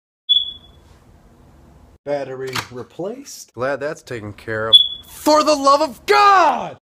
Vine-Elusive Smoke Detector
You think you know which smoke detector is "chirping", oh, but you don't!